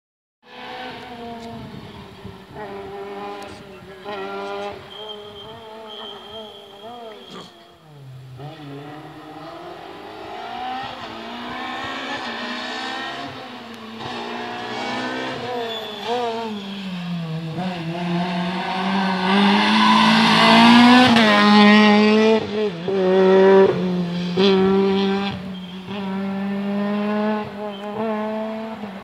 37 Rallye San Martino di Castrozza 2017
Equipaggio n43 Ghegin Luca-Passeri Ivano Renault Clio R3C Scuderia Motor Group